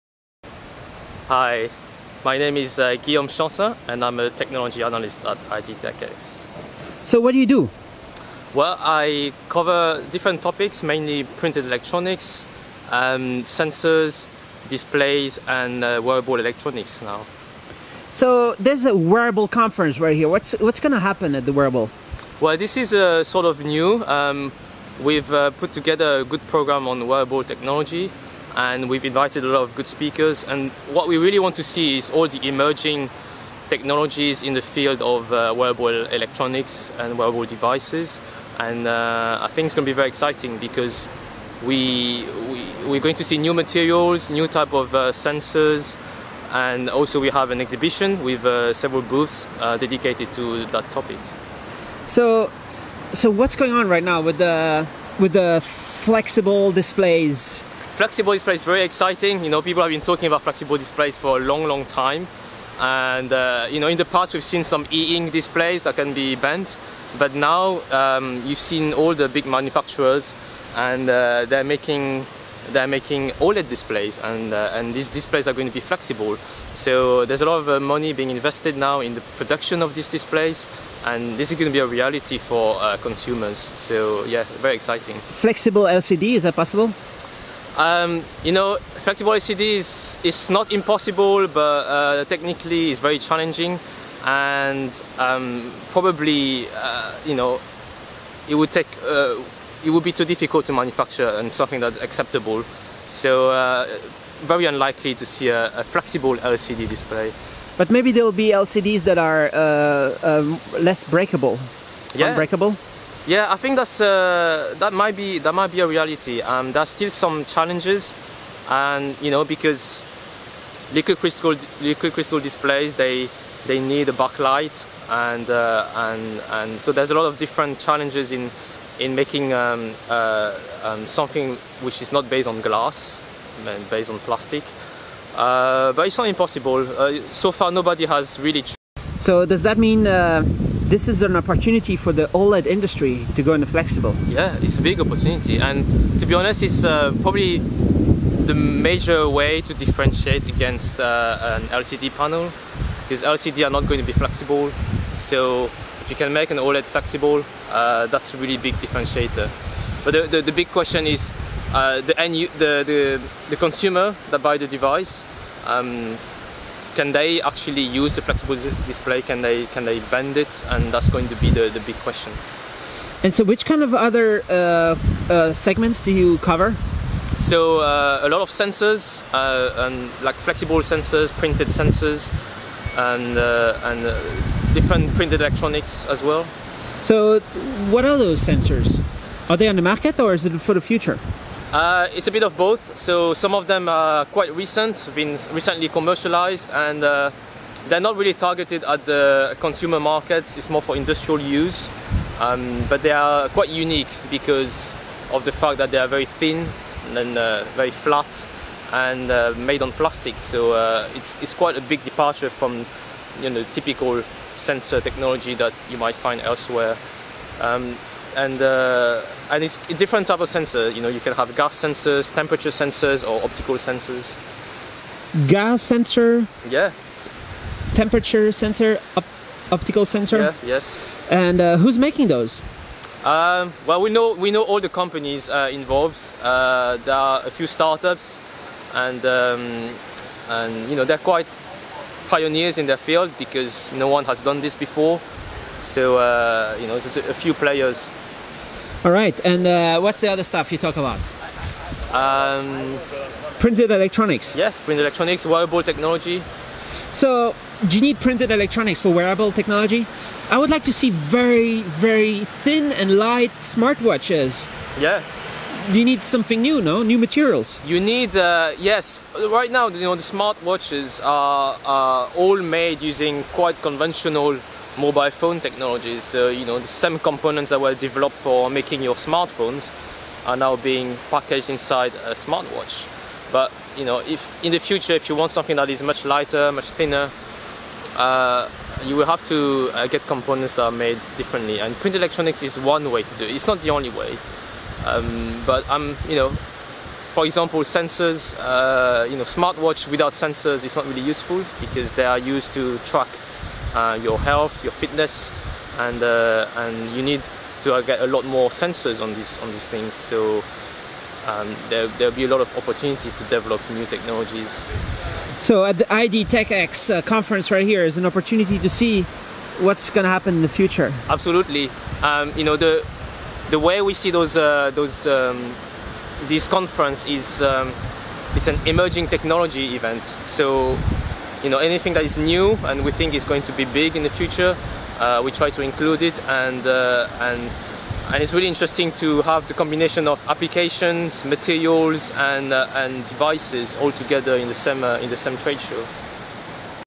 Interview with Guillaume, analyst at IDTechEx
Guillaume Chansin (@GChansin) is one of the analysts from IDTechEx. We meet him in Santa Clara where the company is holding their annual US event. We talk about what we will see during the conference and why printed electronics can help make better wearable devices. He also explains printed sensors, flexible displays, and why we will not see flexible LCD anytime soon.